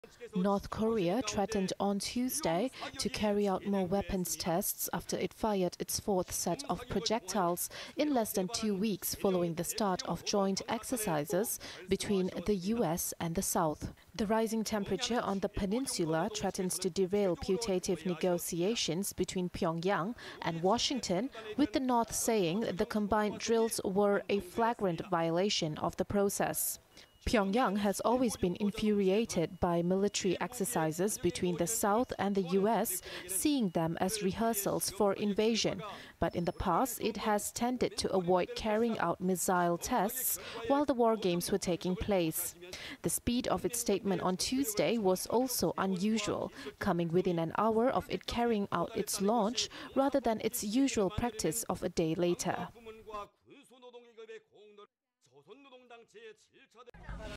Missile Test | North Korea Threatens More Launches After Fourth Test In 12 Days
North Korea threatened on Tuesday to carry out more weapons tests after it fired its fourth set of projectiles in less than two weeks following the start of joint exercises between the US and the South.